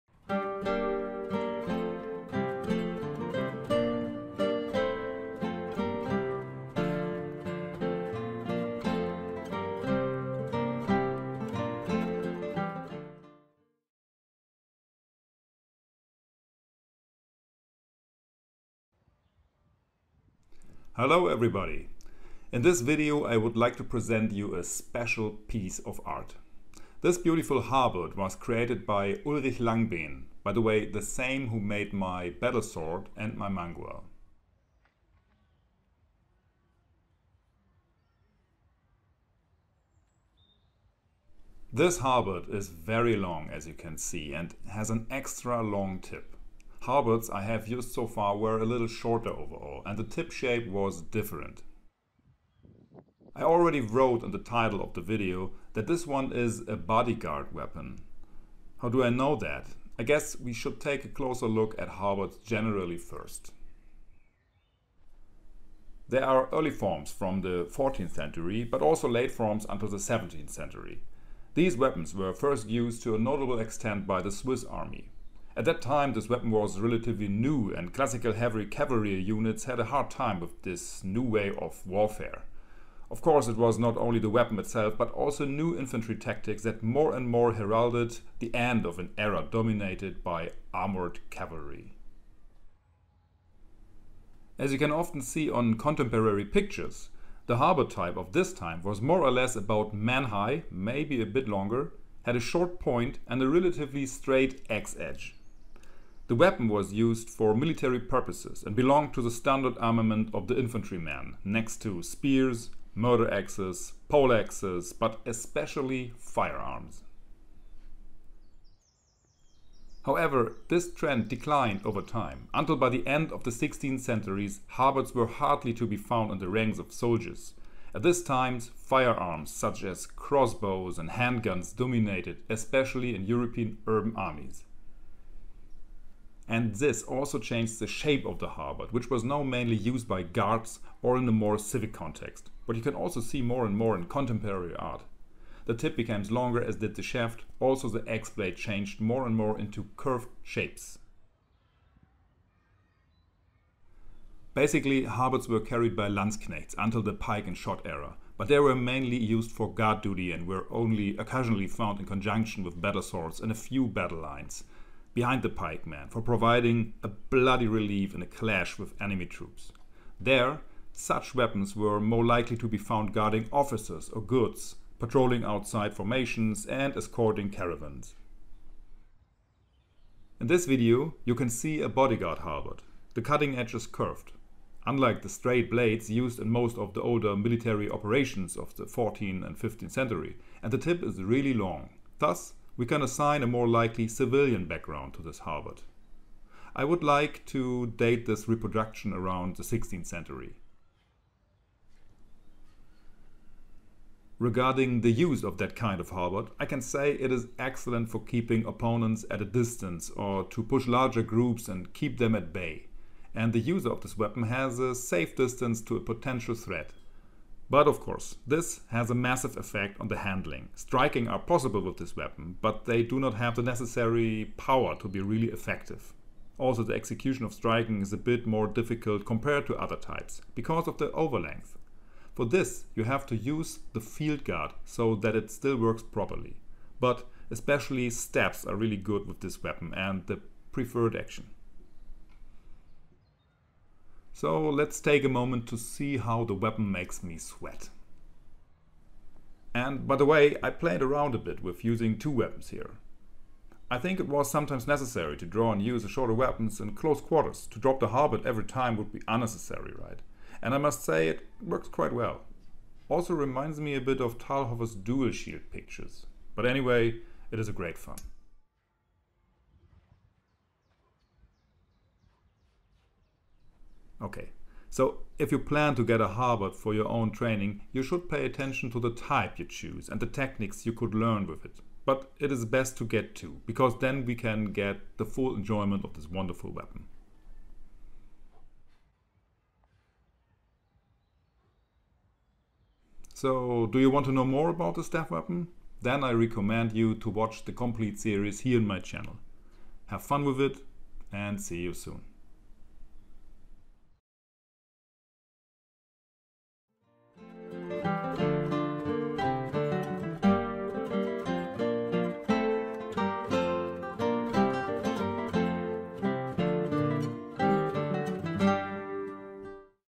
16. century Bodyguard Halberd - Piece of Art
Here I present a particularly beautiful Halberd from the workshop of Ulrich Langbehn. It is a commissioned work and on my special request. The goal was to get a particularly long, thrust emphasized halberd... what came out of it you can you watch in this video. Enjoy!